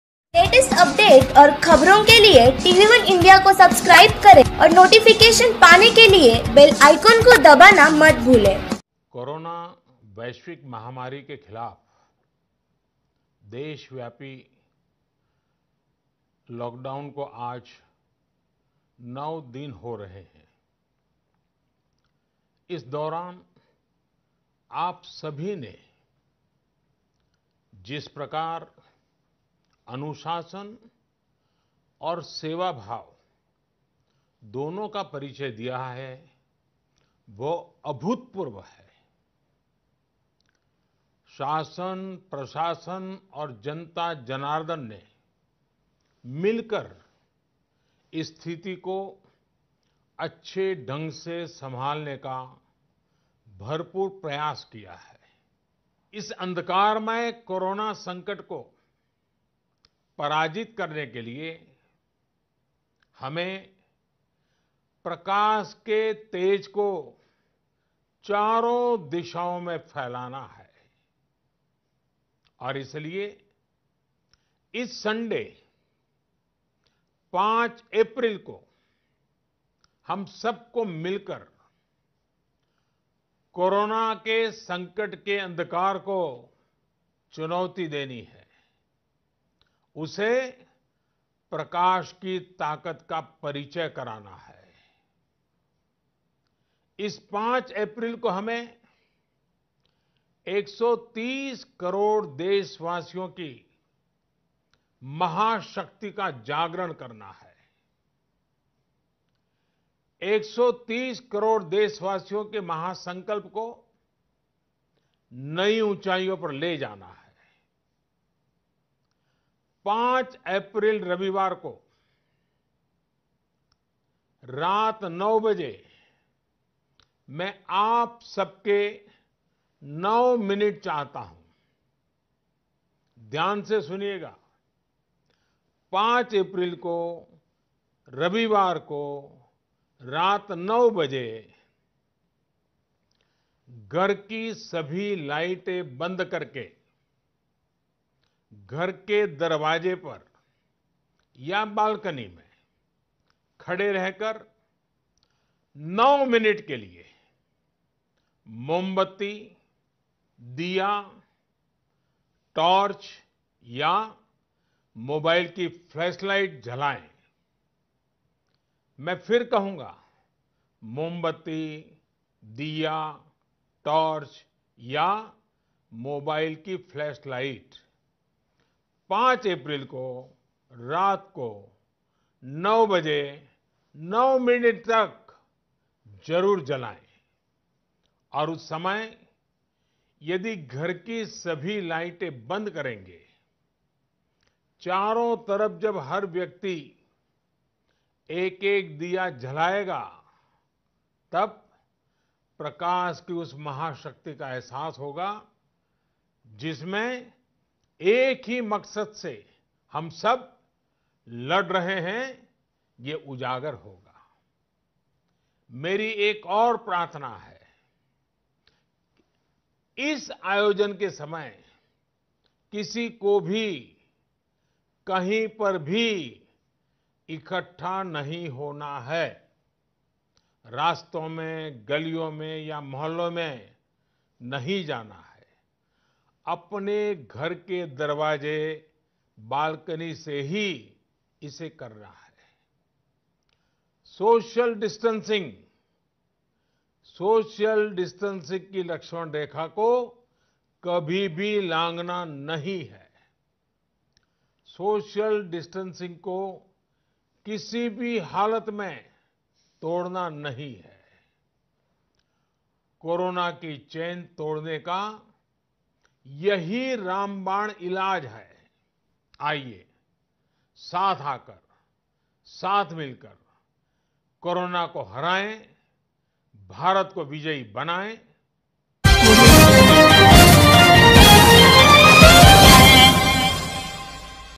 PM मोदी ने बताया कोरोना का रामबाण इलाज | यकीन नहीं तो खुद सुन लीजिए
PM मोदी, कोरोना को हराने लगातार नए प्रयोग कर रहे हैं इस बार उन्होंने रामबाण ईलाज का दावा किया है साथ ही अपील की है कि यदि जनता ने अपनाया लिया तो कोरोना भारत से भाग जाएगा.. Watch it only on .